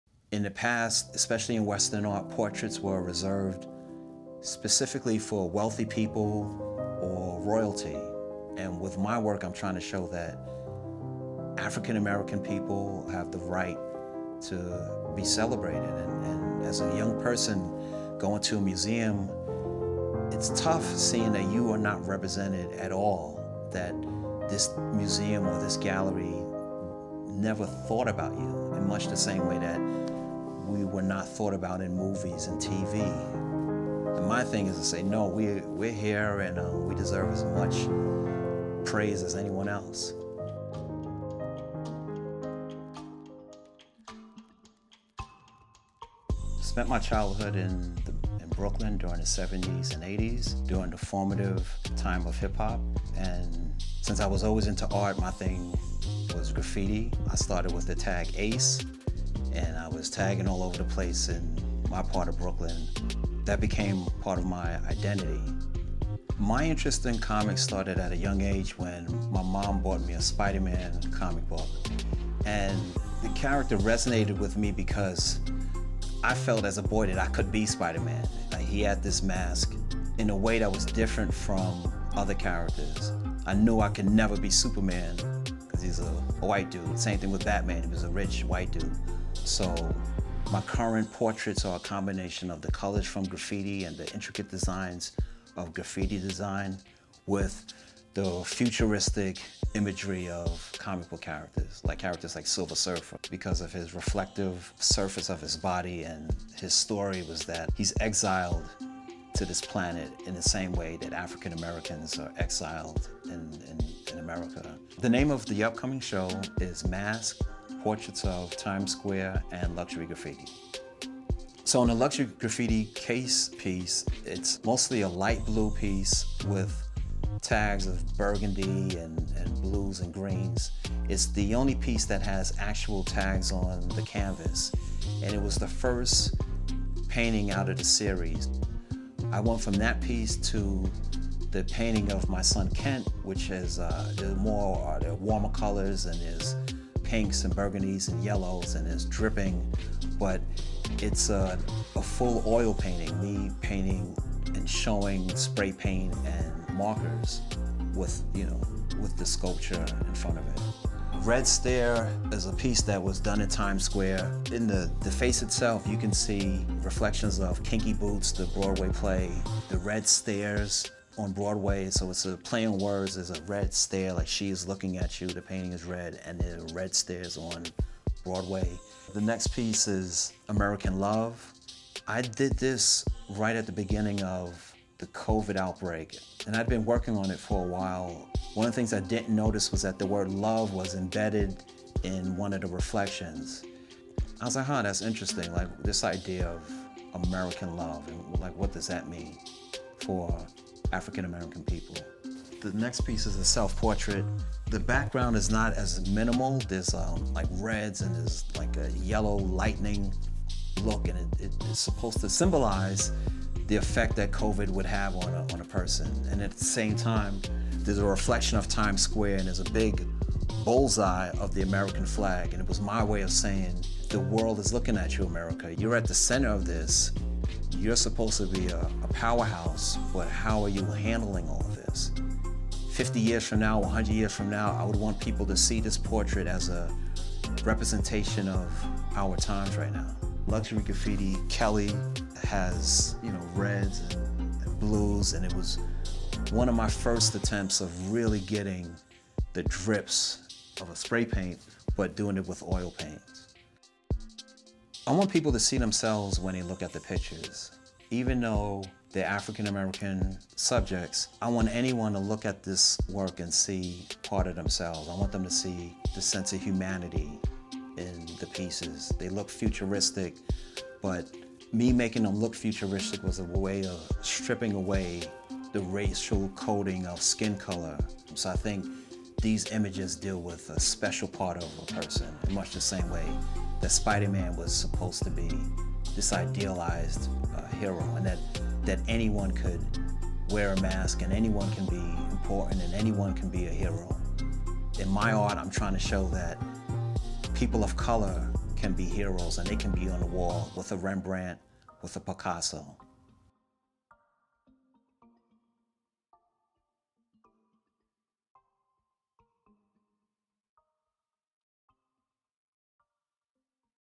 KIP OMOLADE - MASKS: PORTRAITS OF TIMES SQUARE AND LUXURY GRAFFITI
Jonathan LeVine Projects is pleased to present Masks: Portraits of Times Square and Luxury Graffiti, a solo exhibition of new works by Kip Omolade, from September 9—October 4, 2020. Omolade is known for his ongoing series of hyper-realistic oil paintings. His process for each work involves creating a face mold of his subject, casting a resin sculpture and applying a chrome finish. The resulting sculpture serves as a three-dimensional reference for the portrait.